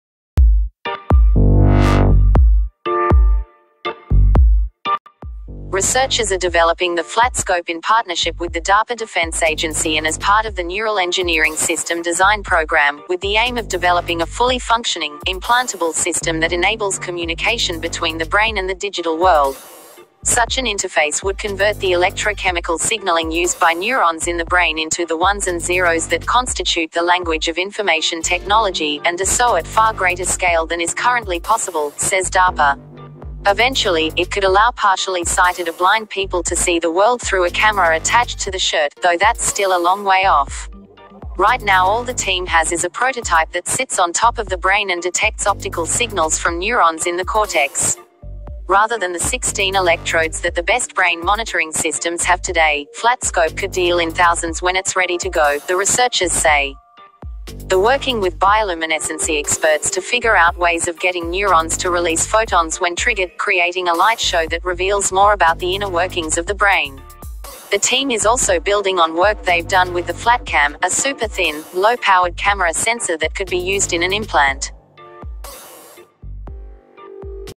FLAT SCOPE CAM BRAIN IMPLANT || ENGLISH- [TECHOMED]
The Researchers are developing the Flat Scope in partnership with the DARPA defence agency as part of the Neural Engineering System Design (NESD) program, with the aim of developing a fully functioning, implantable system that enables communication between the brain and the digital world.

Such an interface would convert the electro chemical signaling used by neurons in the brain into the ones and zeros that constitute the language of information technology, and do so at far greater scale than is currently possible,

Eventually, it could allow partially sighted or blind people to see the world through a camera attached to their shirt, though that's still a long way off.

Right now all the team has is a prototype that sits on top of the brain and detects optical signals from neurons in the cortex.

Rather than the 16 electrodes that the best brain monitoring systems have today, Flat Scope could deal in thousands when it's ready to go, the researchers say.

They're working with bioluminescence experts to figure out ways of getting neurons to release photons when triggered, creating a light show that reveals more about the inner workings of the brain.

The team is also building on work they've done with the FlatCam: a super-thin, low-powered camera sensor that could be used in an implant.

IF U HAVE ANY QUESTION ABOUT THIS U CAN ASK IN COMMENT SECTION..
KEEP LIKING THIS VIDEO AND SHARING..

U CAN ALSO CATCH ME ON-